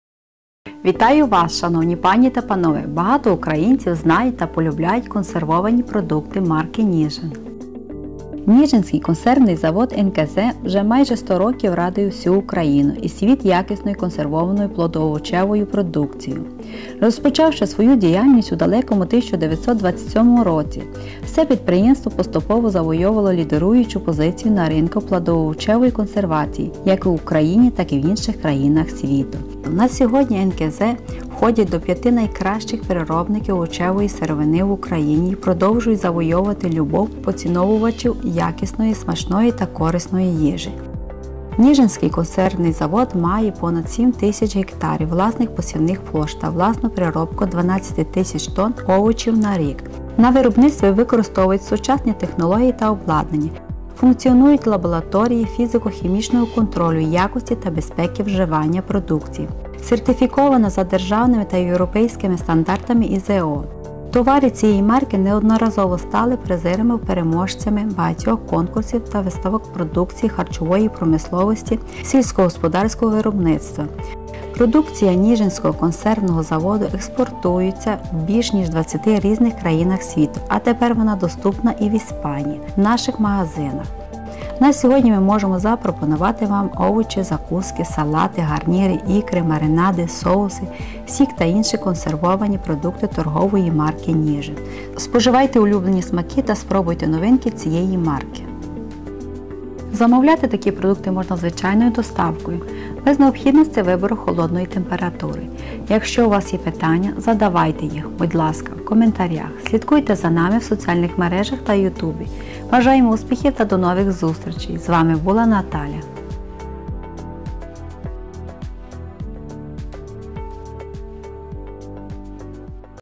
Овочеві закуски, салати, гарніри, ікра, маринади НІЖИН, Україна
Овочеві закуски, салати, гарніри🍎🍆🥬, ікри, маринади, соуси, сік та інші консервовані продукти торгової марки "Ніжин" це корисні та смачні страви до вашого столу!😋